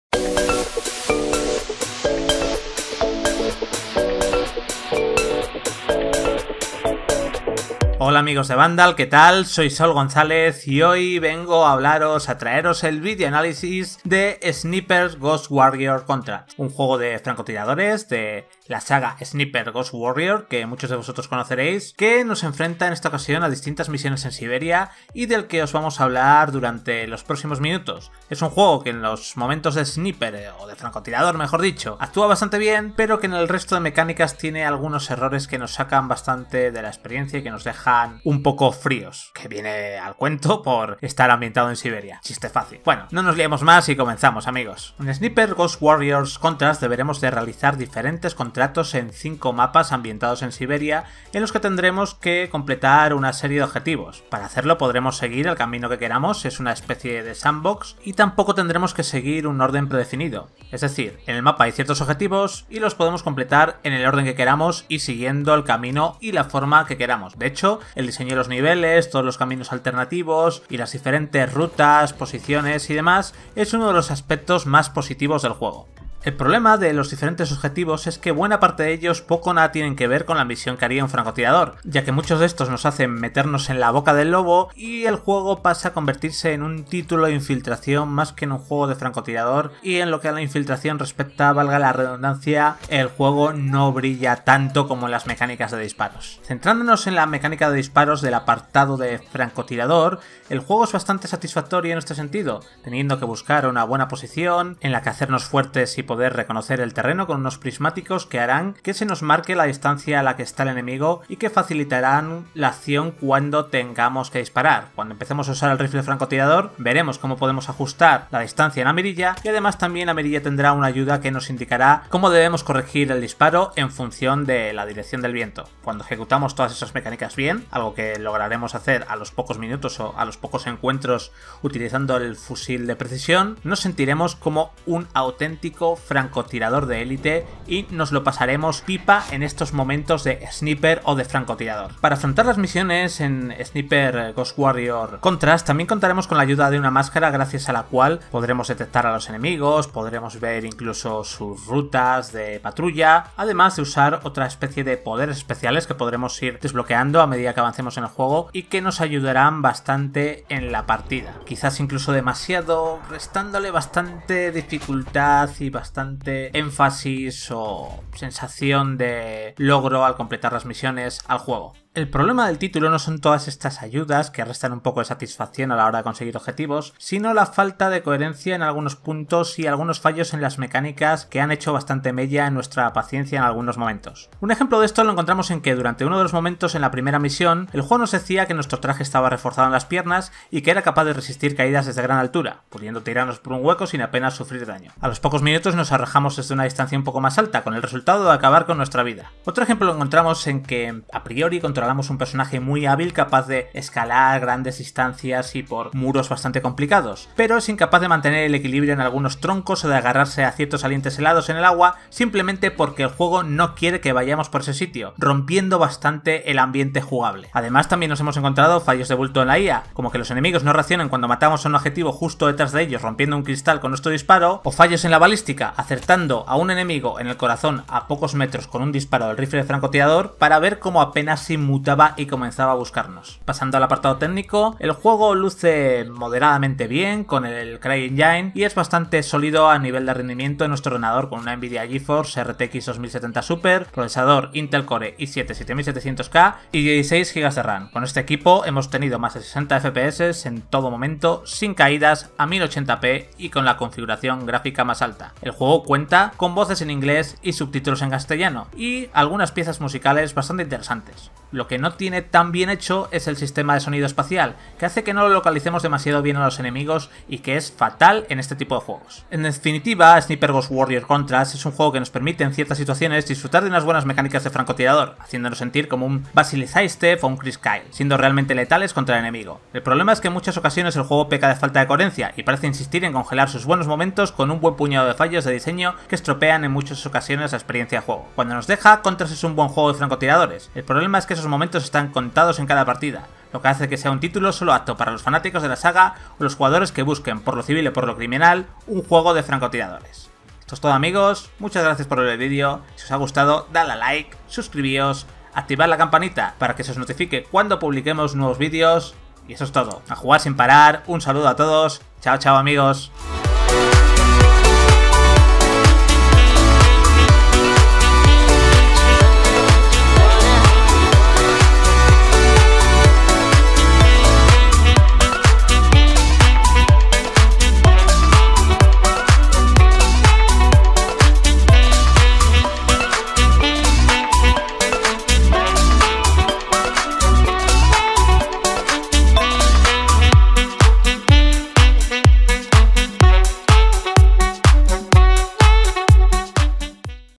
ANÁLISIS SNIPER GHOST WARRIOR CONTRACTS (PC, XBOne, PS4) Contratos de francotirador helados
Aviso: Este análisis se realizó sin el parche del día 1, que ya está disponible desde hace unas horas y que soluciona multitud de errores, como os explicamos en esta noticia

Análisis Sniper Ghost Warrior Contracts, la nueva entrega de la saga de acción táctica de CI Games,que se pone a la venta este viernes 22 de noviembre en PS4, Xbox One y PC. La saga Sniper Ghost Warrior vuelve a la carga con una entrega que nos intenta ofrecer grandes momentos como francotirador que quedan congelados por algunos graves fallos de diseño.